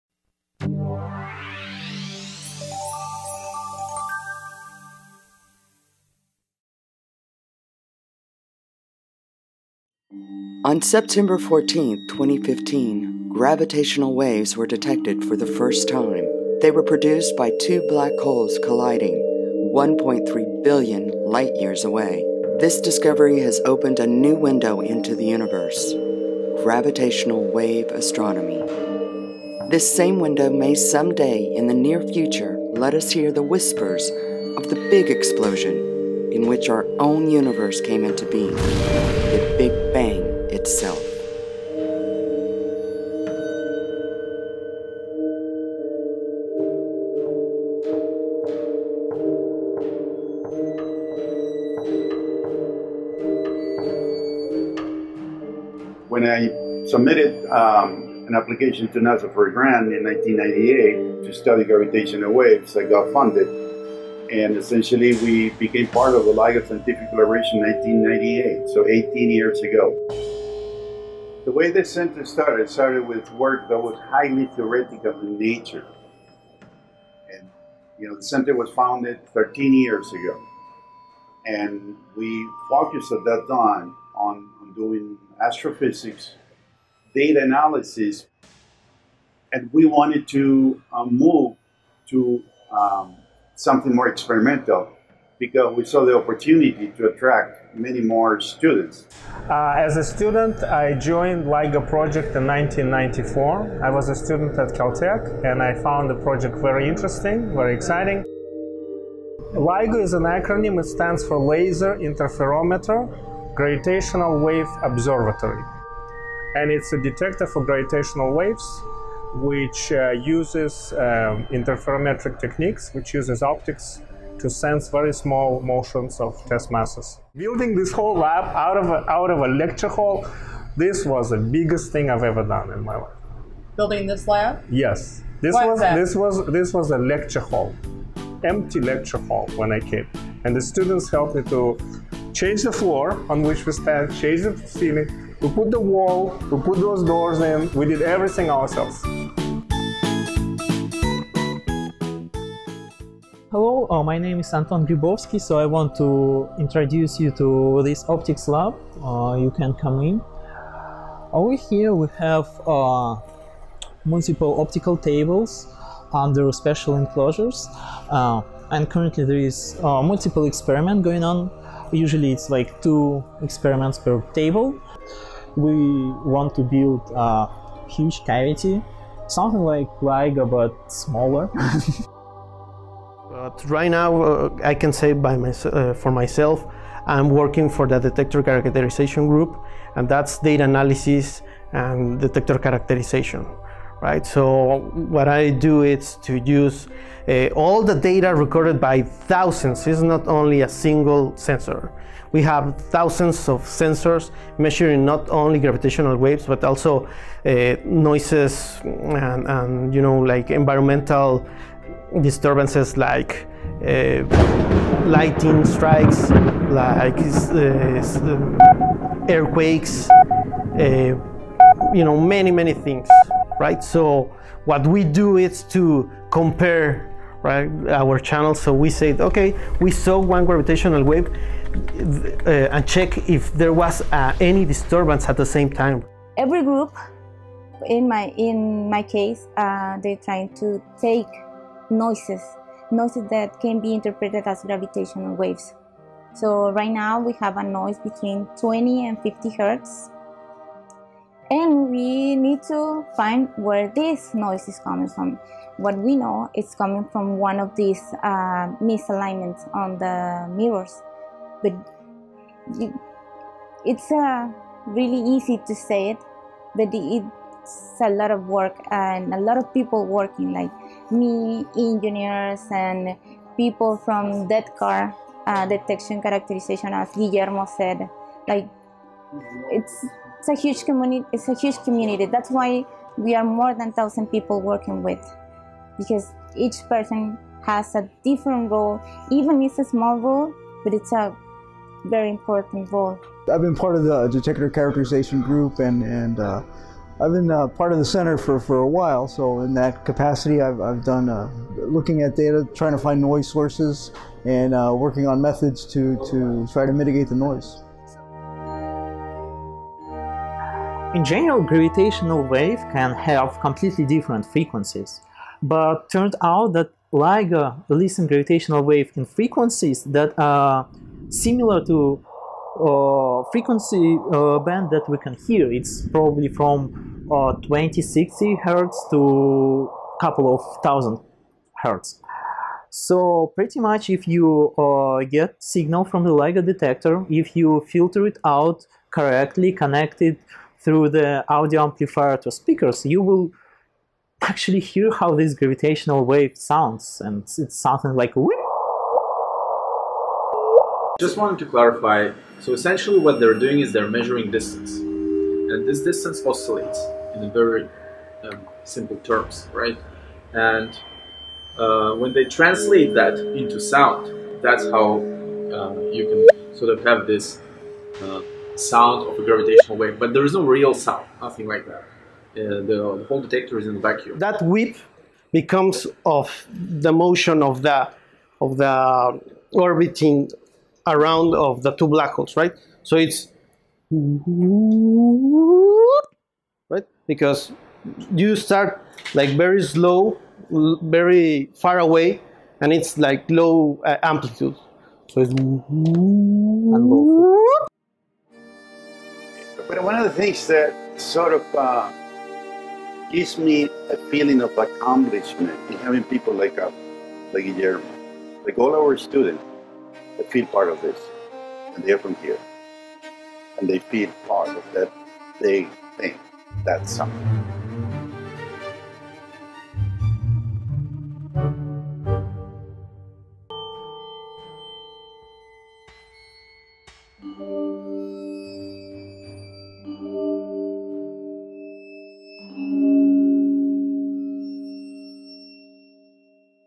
UTRGV Center for Gravitational Waves "The Amazing Discovery"
This video called, "The Amazing Discovery", is about UTRGV Center for Gravitational Waves Astronomy's role in the discovery of gravitational waves with the National Science Foundation's LIGO Scientific Collaboration.